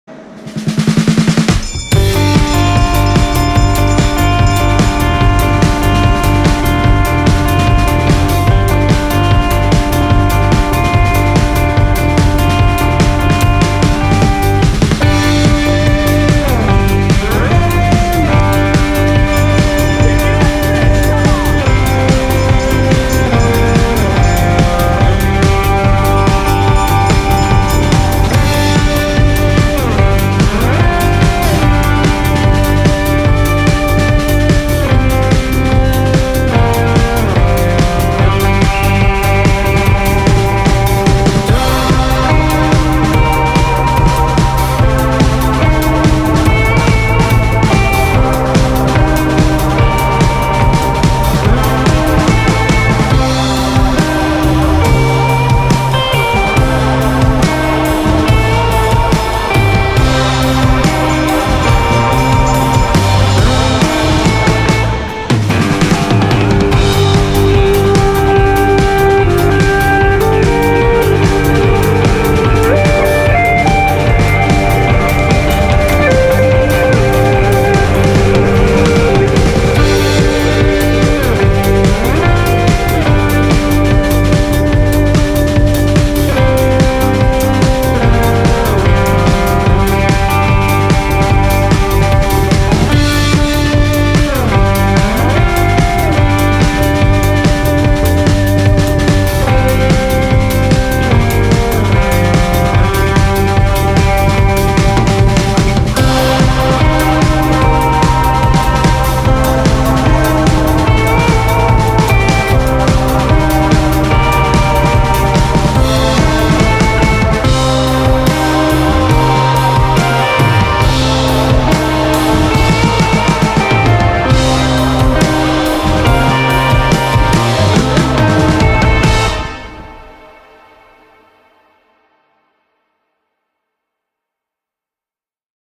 Winter Track League At The Geraint Thomas National Velodrome Of Wales 03.12.19
A few clips from track league on 03.12.19.